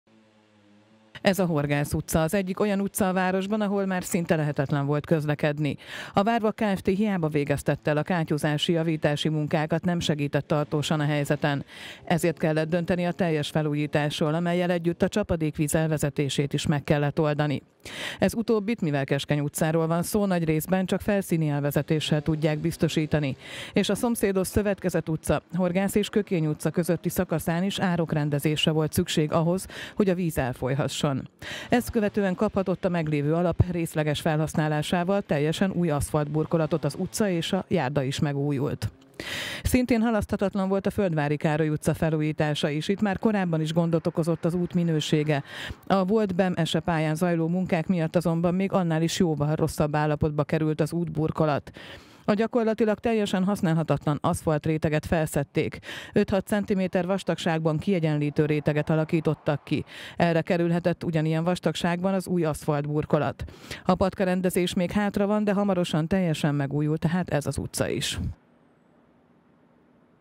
Megújuló utcák
Két olyan út is megújul a városban, amely gyakorlatilag már járhatatlan volt. A Horgász utca és a Földváry Károly utca kap új aszfaltborítást. Mindkettő a költségvetésben tervezett útfelújítási program része.